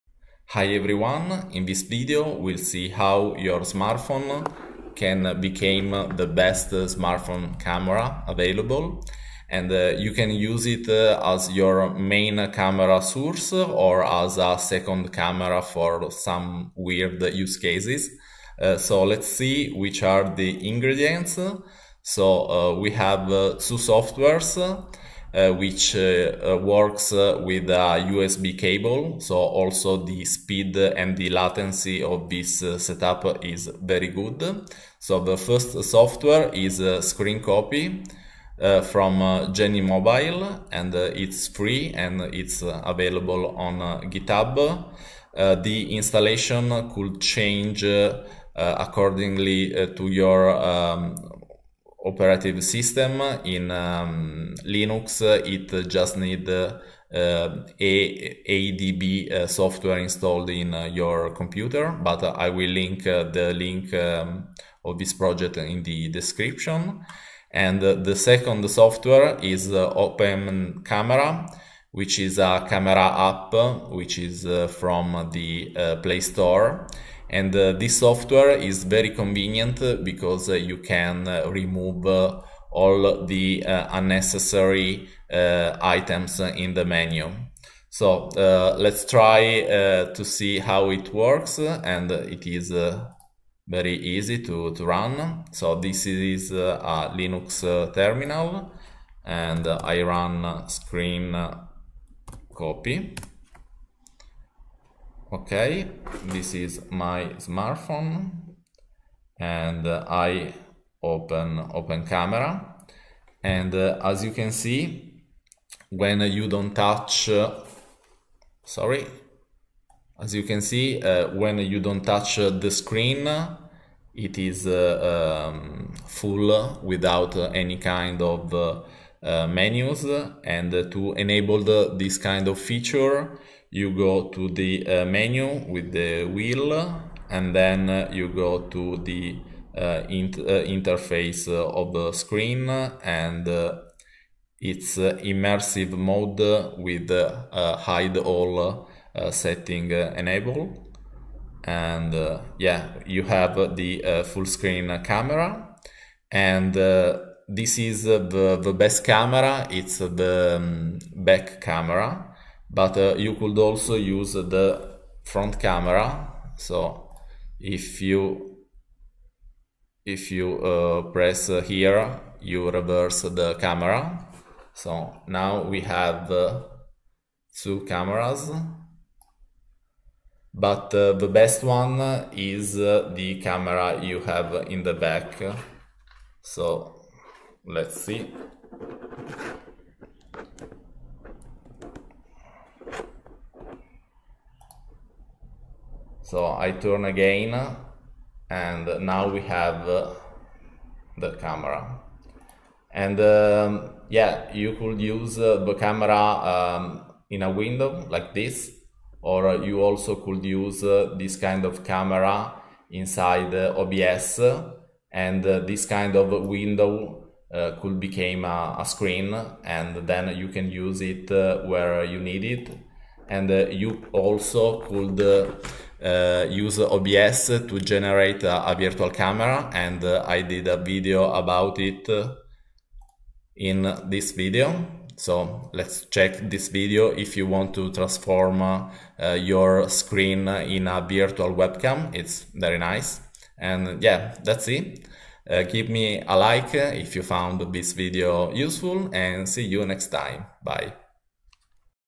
🎥 HOW TO USE THE SMARTPHONE AS YOUR BEST WEBCAM FOR OBS and LIVE STREAMING
In this video we'll see how your smartphone can be used as webcam